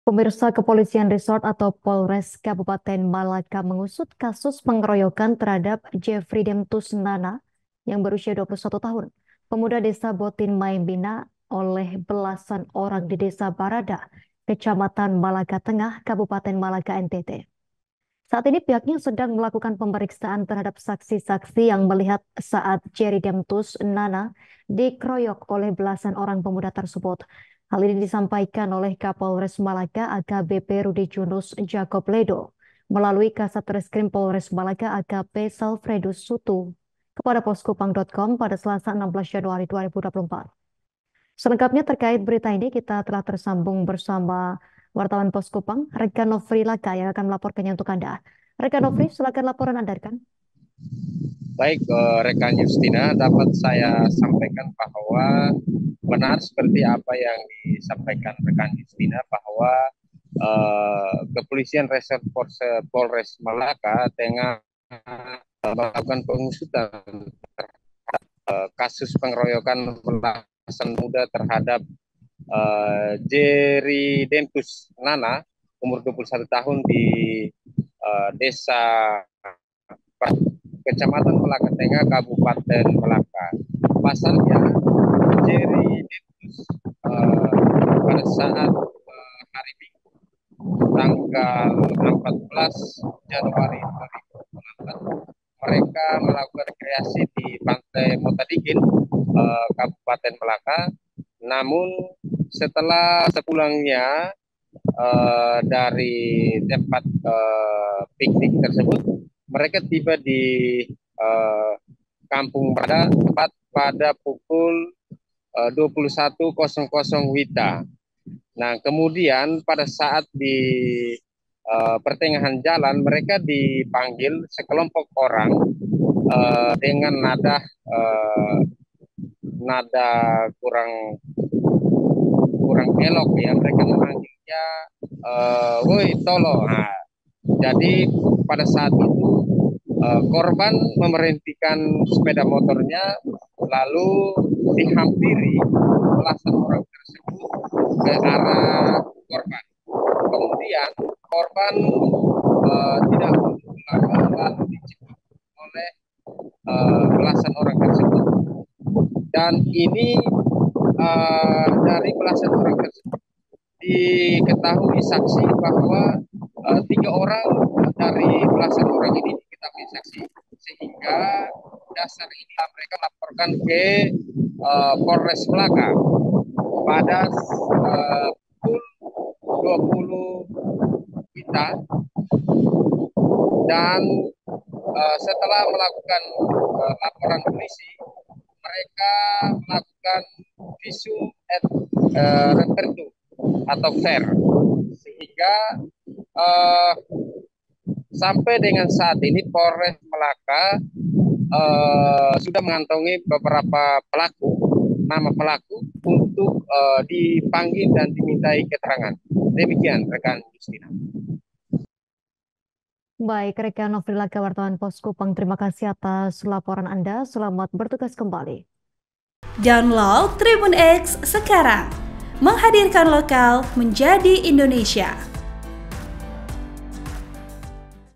Polres Malaka Usut Kasus Pengeroyokan Pemuda Botin Maemina, Periksa Saksi yang Lihat Kejadian
Laporan wartawan Pos Kupang, Nofry Laka
TRIBUN-VIDEO.COM -  Kepolisian Resort atau Polres Kabupaten Malaka mengusut kasus pengeroyokan terhadap Jefridemtus Nana (21), pemuda Desa Botin Maemina oleh belasan orang di Desa Barada, Kecamatan Malaka Tengah, Kabupaten Malaka, NTT.

Saat ini, pihaknya sedang melakukan pemeriksaan terhadap saksi-saksi yang melihat saat Jeridemtus Nana dikeroyok oleh belasan orang pemuda tersebut.

Hal ini disampaikan oleh Kapolres Malaka, AKBP Rudi Junus Jakob Ledo, SH.,SIK melalui Kasat Reskrim Polres Malaka, AKP Salfredus Sutu kepada POS-KUPANG.COM, Selasa 16 Januari 2024.(*)